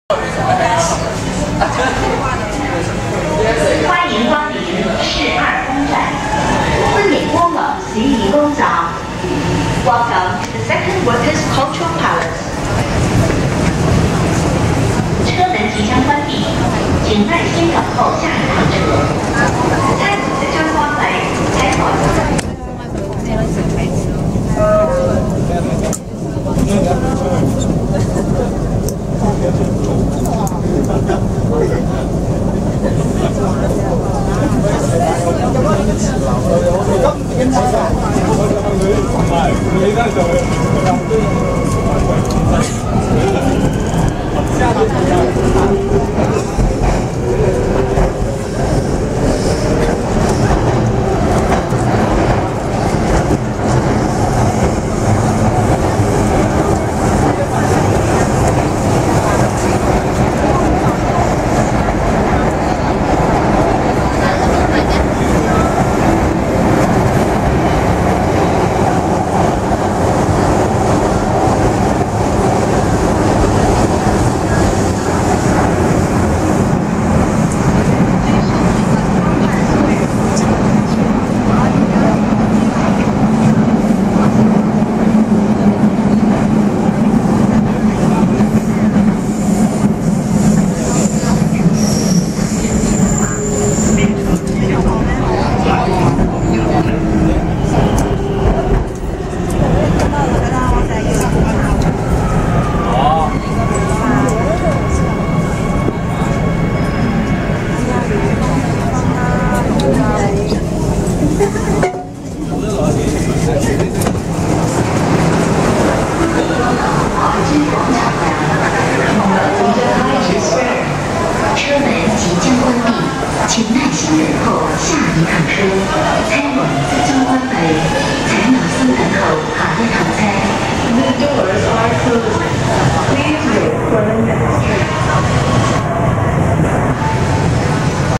廣州地鐵二號線 (市二宮 - 海珠廣場) Guangzhou Metro Line 2 in MOVIA 456 Train
市二宮站仍用較舊式的駐站廣播